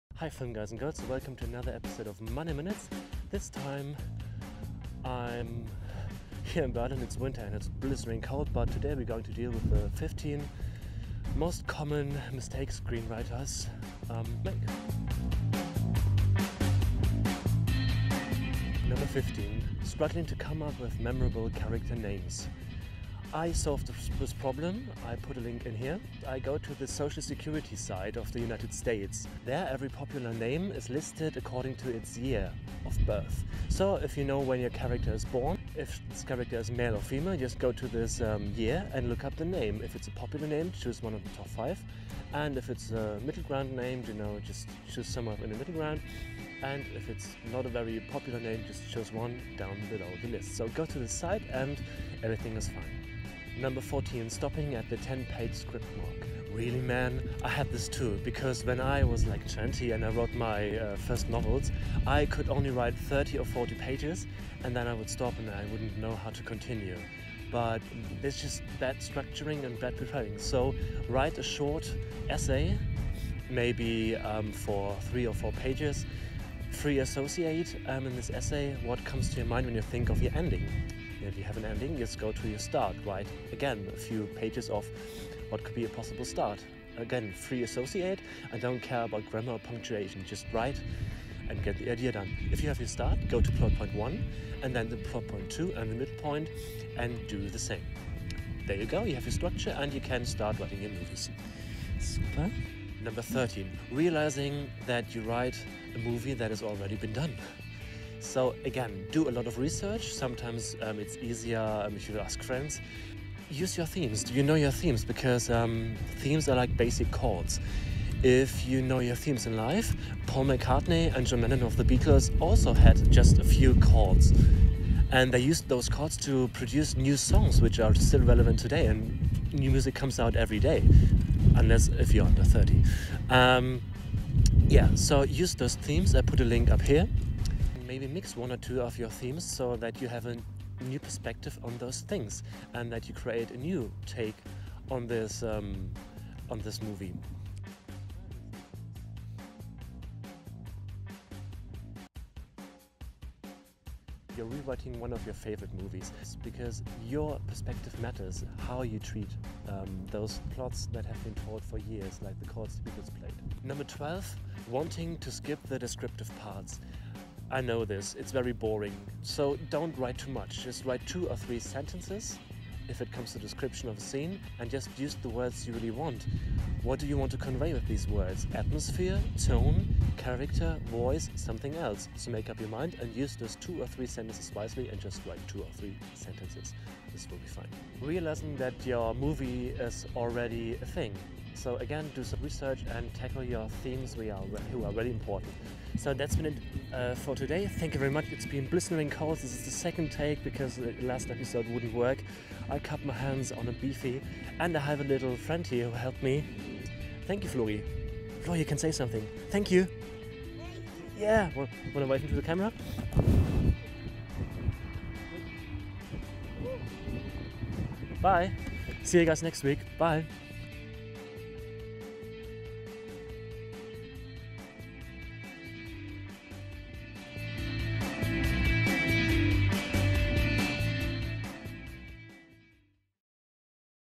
"15 mistakes every screenwriter makes & 15 solutions" 1/2 Monday Minutes #33 film vlog
Heya guys and girls,
screenwritng is complex and there are a lot of problems you can encounter. Here are my top fifteeen  traps you might stumble upon -
and the solutions, of course.
The last part will be published next week, maybe you have some questions until then, so please comment ;)

Have fun and be sure to check out the other videos, too ;)

Greetings from Berlin
James

Concept/director/host/text/camera/editing/mixing/grading: James A. Delancey